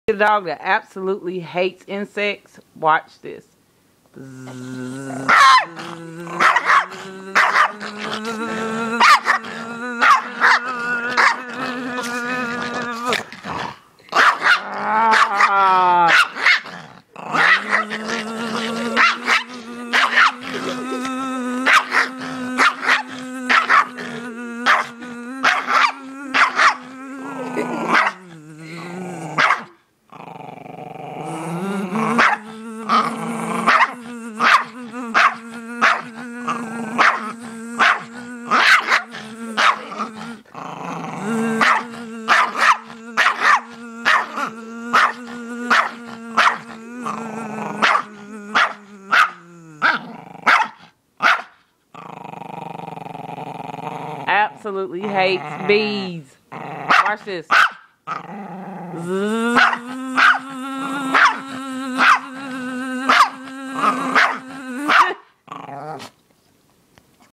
The dog that hates bees buzzing
This dogs goes nuts whenever he hears a buzzing noise.